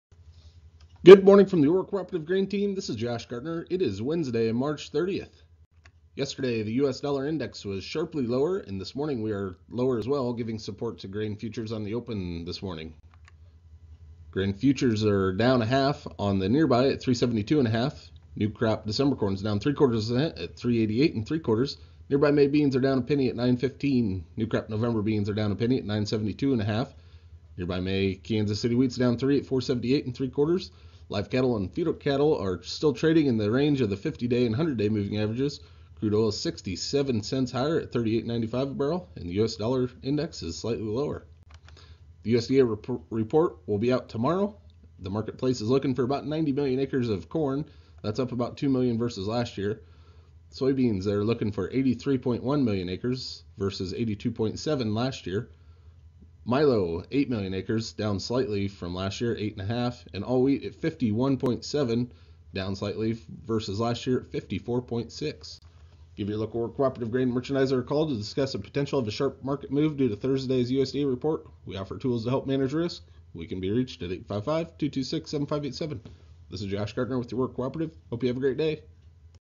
3 30 16 Aurora Cooperative Grain Market Commentary
The Lower US Dollar Index provided support for CME Grain futures on Tuesday, follow thru to the downside is supporting values this morning as well.  The trade isn't expecting much excitement today as the trade positions for Thursdays USDA Quarterly Stocks report along with the Planting Intentions report.